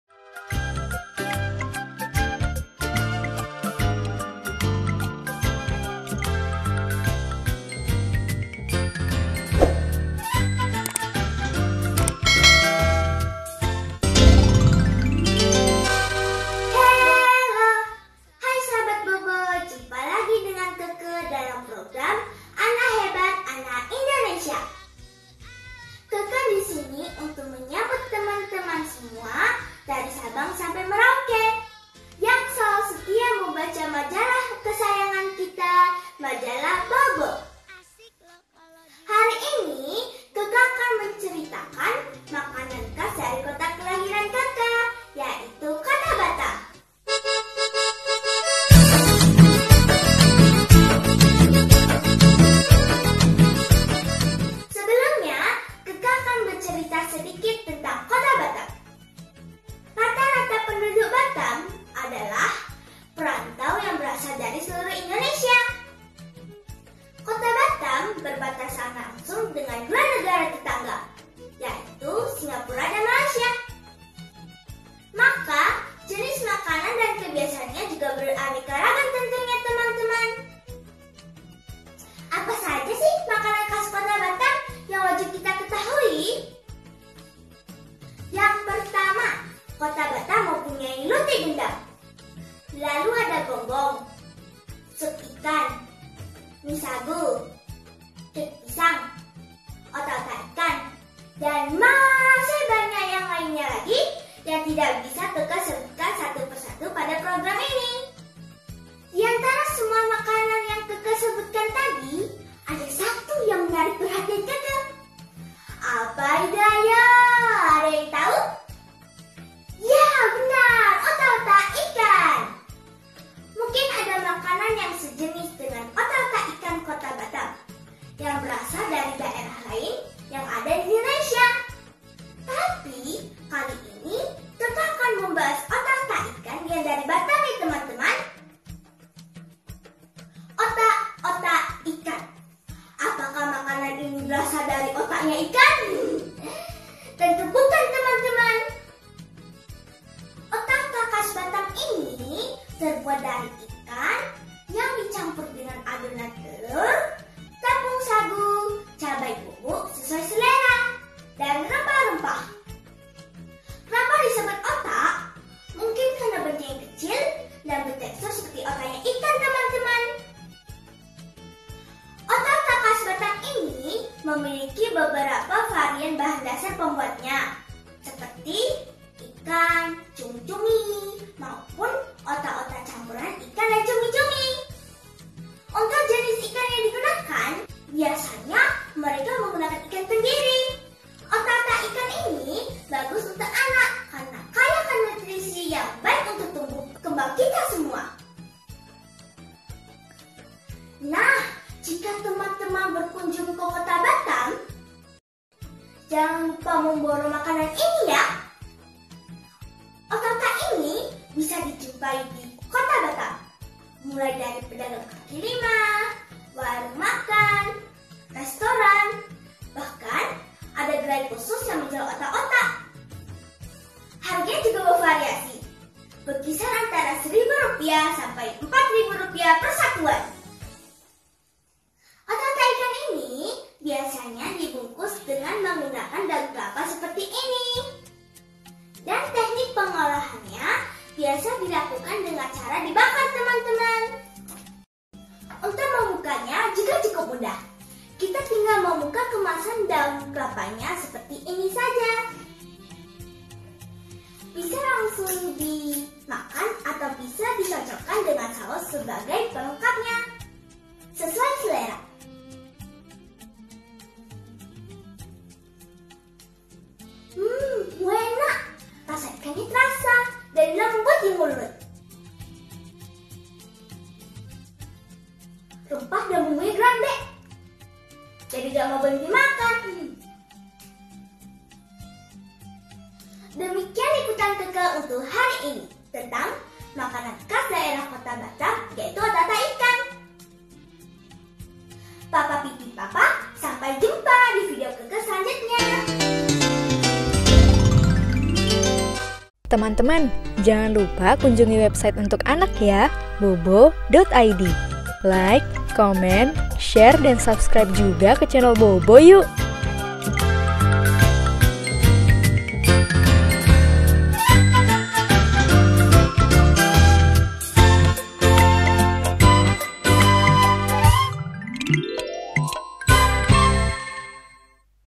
Fakta Menarik - Makanan Khas Kota Batam - Reporter Cilik Bobo - Angellysta Belva Milena Niari
Setelah mendapat pelatihan reporter cilik selama 2 hari, 40 finalis reporter cilik diberi tugas untuk membuat karya video.
Berikut ini hasil karya Angellysta Belva Milena Niari dari kota Batam.
Yuk, lihat videonya!

Assisten Editor: Glent Bonefasio
Video Editor & Videografer: Inggar Rivaldi, Vinnie Januarty Wongso, Aria Pratama
Creative Art : Revydia Darmawan, Donny Suryanto, Anastasia A.C.